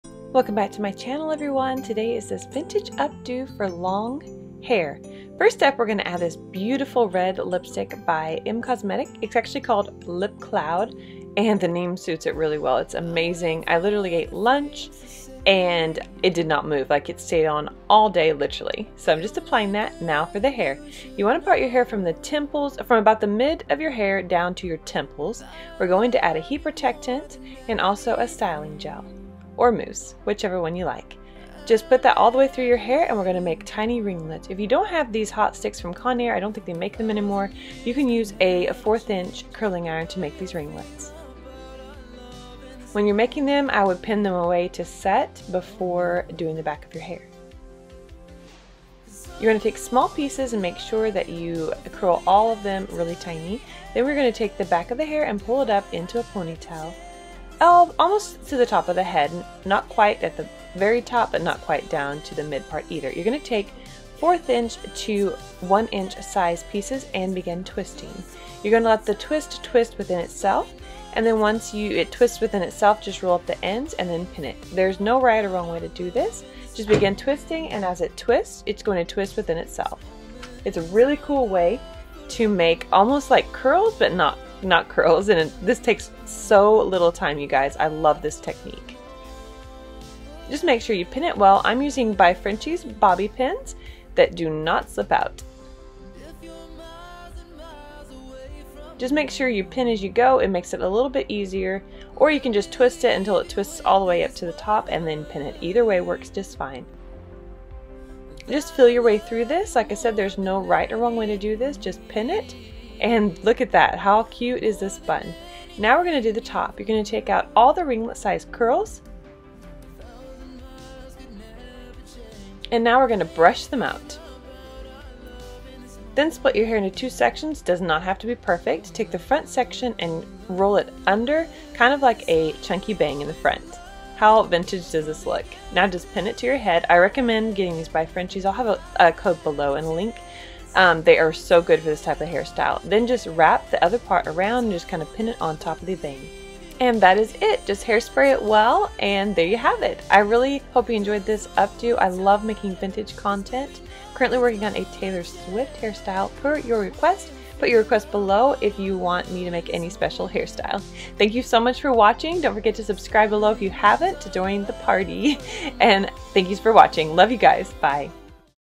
Vintage Hair Tutorial
Today is a Vintage Hair tutorial you can do on your self!

I love making Vintage content it’s one of my favorites,
Do you like Vintage hairstyles? Would you wear this style?

Here is the link to buy 👇🏻